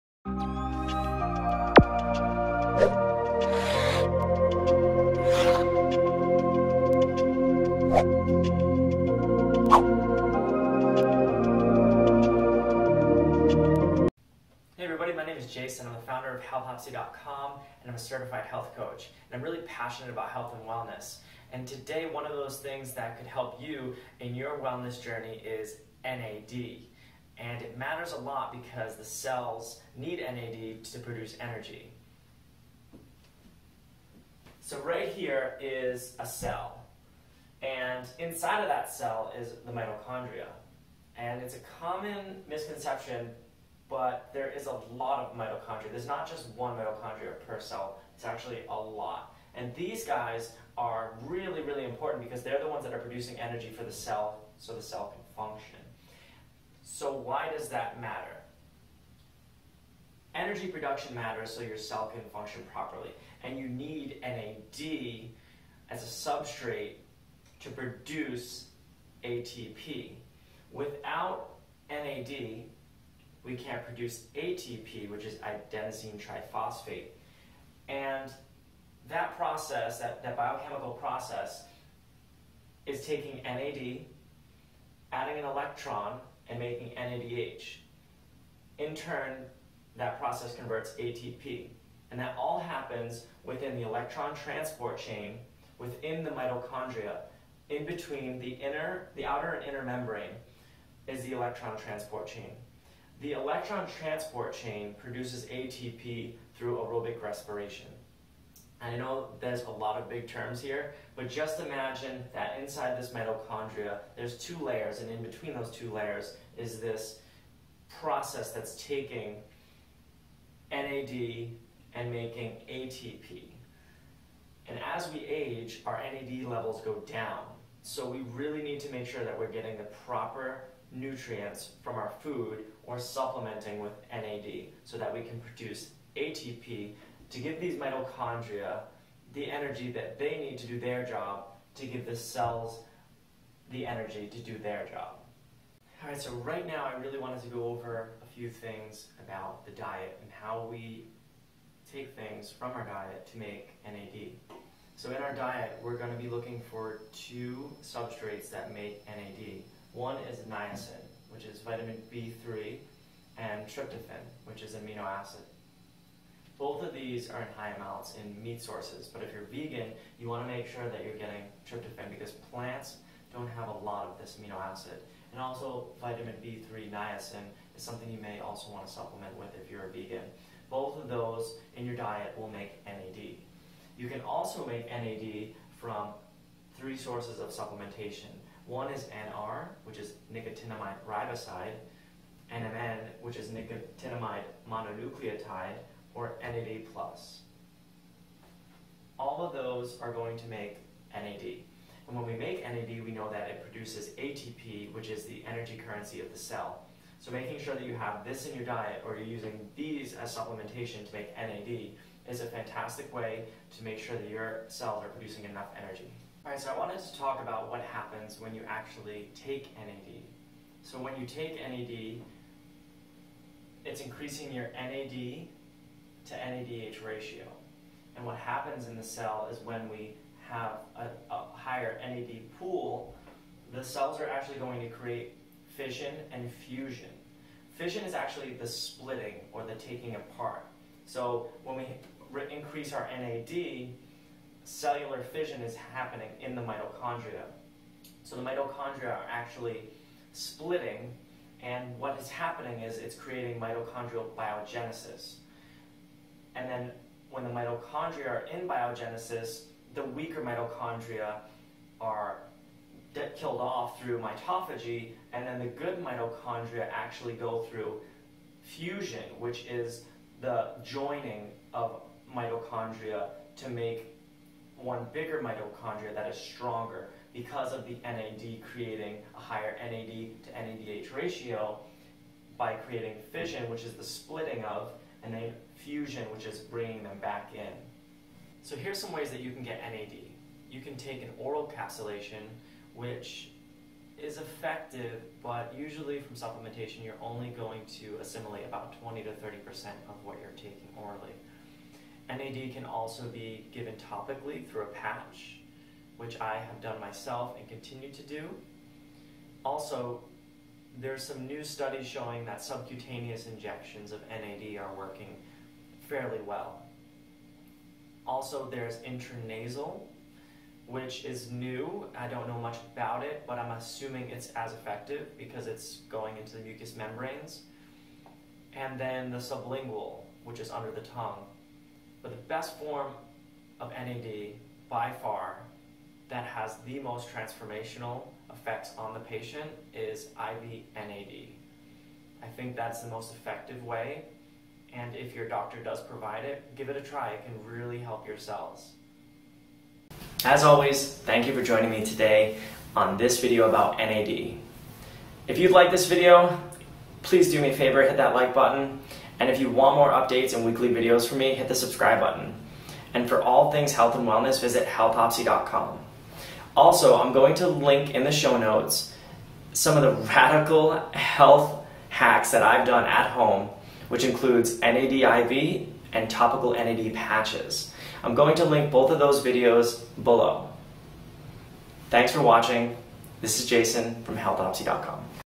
Everything You Need To Know About NAD Therapy & NAD Supplements
My name is Jason,
I love all things health and wellness. Today I want to talk to you about NAD and its role in the mitochondria. NAD is a supercritical molecule to produce ATP in the cells. You need these two important substrates called niacin and tryptophan to make NAD. Or you can supplement, use a nad patch or get and NAD IV.
These are the benefits of NAD
Supports cellular energy production
Supports brain function
Supports healthy metabolic function
Supports normal liver function
 Promotes healthy aging

The BEST NAD NR supplement